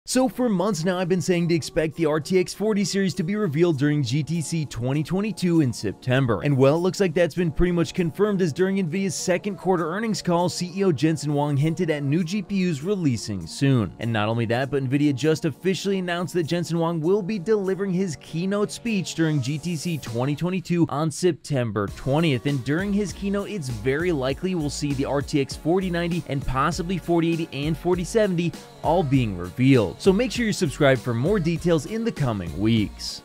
You Might Want to See This…
So for months now I've been saying to expect the RTX 40 series to be revealed during GTC 2022 in September, and well it looks like that's been pretty much confirmed as during Nvidia’s second quarter earnings call CEO Jensen Huang hinted at new GPUs releasing soon.

And not only that, but Nvidia just officially announced that Jensen Huang will be delivering his Keynote speech during GTC 2022 on September 20th, and during his keynote it's VERY likely we will see the RTX 4090 and possibly 4080 and 4070 all being revealed.